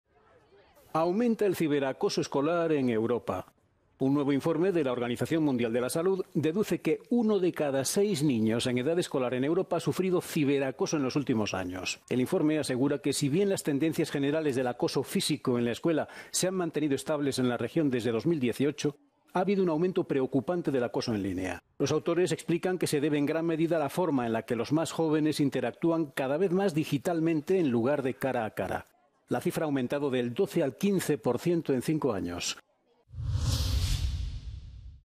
Uno de cada seis niños en edad escolar sufre ciberacoso en Europa según la OMS
La Oficina Regional para Europa de la Organización Mundial de la Salud publicó el segundo volumen del estudio Comportamiento de salud en niños en edad escolar, que se centra en los patrones de actuación digital.